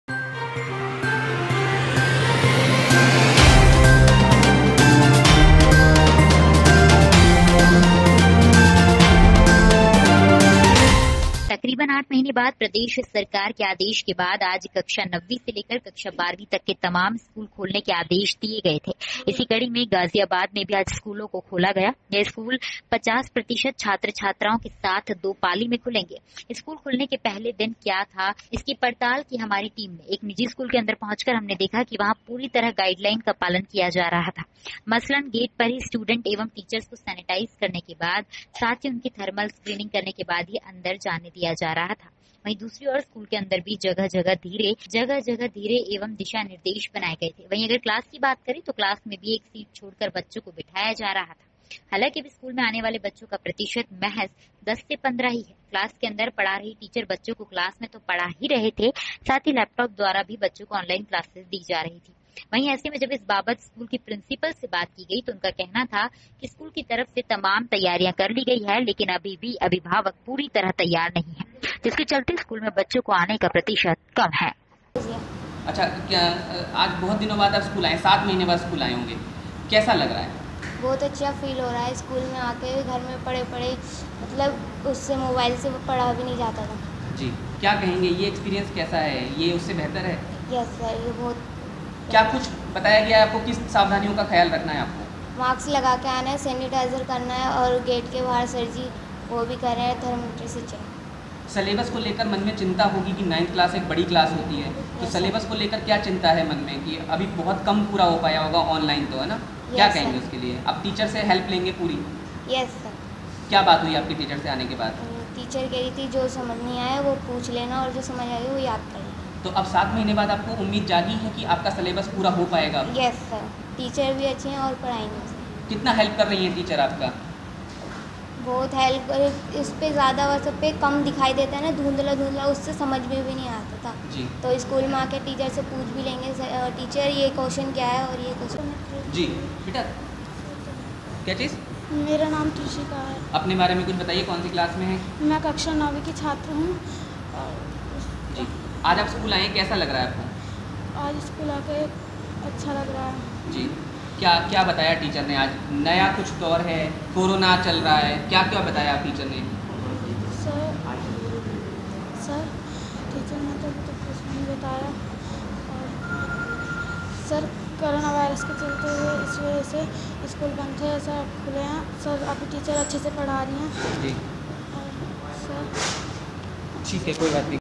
गाजियाबाद में भी स्कूल खोले गये
तकरीबन 8 महीने बाद प्रदेश सरकार के आदेश के बाद आज कक्षा 9 से लेकर कक्षा 12वी तक के तमाम स्कूल खोलने के आदेश दिए गए थे। इसी कड़ी में गाजियाबाद में भी आज स्कूलों को खोला गया यह स्कूल 50% छात्र छात्राओं के साथ दो पाली में खुलेंगे। स्कूल खुलने के पहले दिन क्या था स्कूलों का सूरत हे हाल इसकी पड़ताल की हमारी टीम ने एक निजी स्कूल के अंदर पहुंचकर हमने देखा कि वहां पूरी तरह गाइडलाइन का पालन किया जा रहा था मसलन गेट पर ही स्टूडेंट एवं टीचर्स को सेन्टाइज़ करने के बाद साथ ही उनकी थर्मल स्क्रीनिंग करने के बाद ही अंदर जाने दिया जा रहा था वहीं दूसरी ओर स्कूल के अंदर भी जगह-जगह धीरे एवं दिशा निर्देश बनाए गए थे। वहीं अगर क्लास की बात करें तो क्लास में भी एक सीट छोड़कर बच्चों को बिठाया जा रहा था हालांकि अभी स्कूल में आने वाले बच्चों का प्रतिशत महज 10 से 15 ही है। क्लास के अंदर पढ़ा रही टीचर बच्चों को क्लास में तो पढ़ा ही रहे थे साथ ही लैपटॉप द्वारा भी बच्चों को ऑनलाइन क्लासेज दे रहे थे वही ऐसे में जब इस बाबत स्कूल की प्रिंसिपल से बात की गई तो उनका कहना था की स्कूल की तरफ से तमाम तैयारियां कर ली गई है लेकिन अभी भी अभिभावक पूरी तरह तैयार नही है जिसके चलते स्कूल में बच्चों को आने का प्रतिशत कम है।